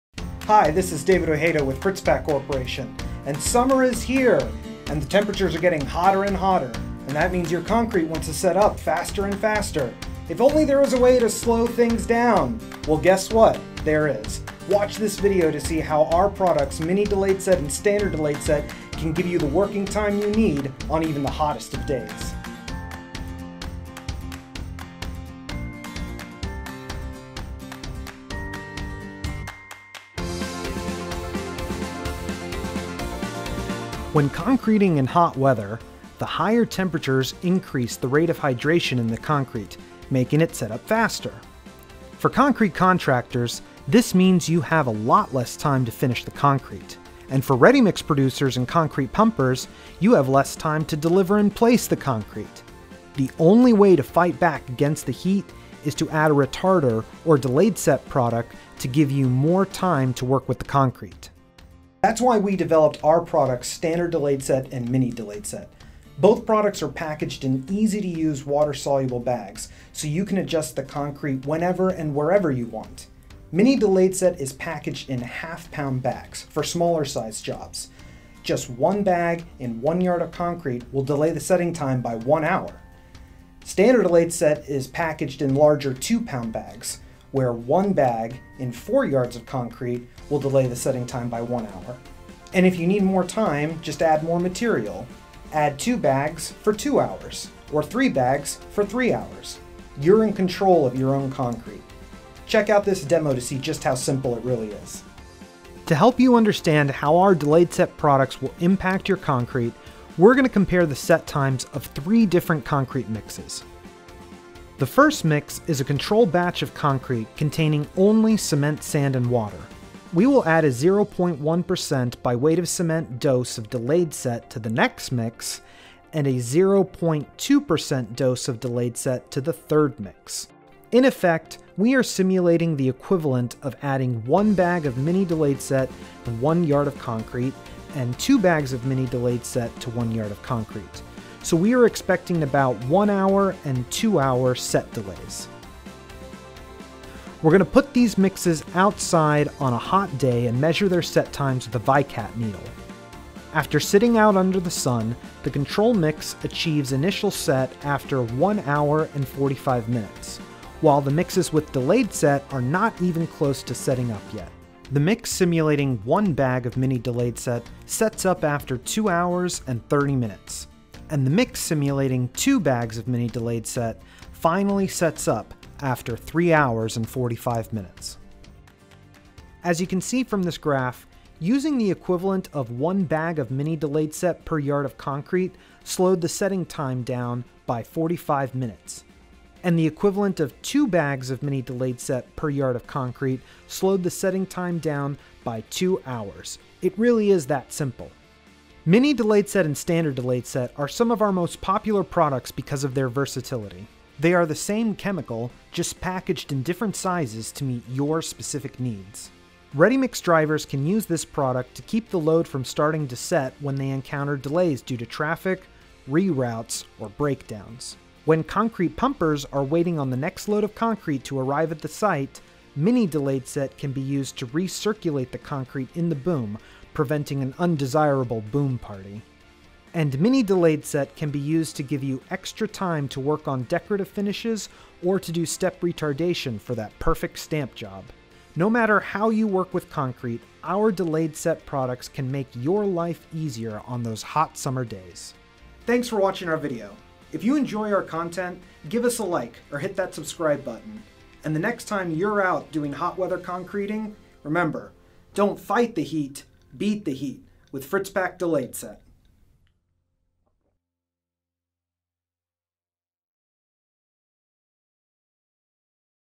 Improve Hot Weather Concrete with Delayed Set Concrete Admixtures
00:00 Introduction
00:34 Why does concrete set up faster in hot weather?
01:04 Fritz-Pak's Delayed Set Admixtures
01:45 Lab Test: The Set Up
02:33 Lab Test: The Results
03:19 When to Use a Delay Set Admixtures
04:05 visit fritzpak.com today

Summer temperatures are here!  And if you need a product to help combat fast-setting concrete, nothing beats the set delayed concrete admixtures from Fritz-Pak Corporation. Whether you’re working on a big job that requires a lot of finishing or you have a small job that needs smoothing out, using our set delay products is the fastest, easiest way to ensure you have the time you need to finish your concrete.
Hot weather concreting is one of the most challenging things you encounter on the job as a concrete worker. This is because as the temperature rises, the hydration process speeds up – making your concrete set up before you’re ready.  This can make placing decorative stamped concrete almost impossible and is going to result in an unhappy customer.
Fritz-Pak’s delayed set admixtures are premeasured and packaged in water-soluble bags.  So all you have to do is add them to the truck and mix.  You can add them at the plant to prevent the concrete from setting up on the road and then add more at the job site to ensure a beautiful concrete finish.
Whatever hot weather concreting project you’re working on, don’t get caught unprepared.
Shout out to Brundage Bone for providing the drone footage.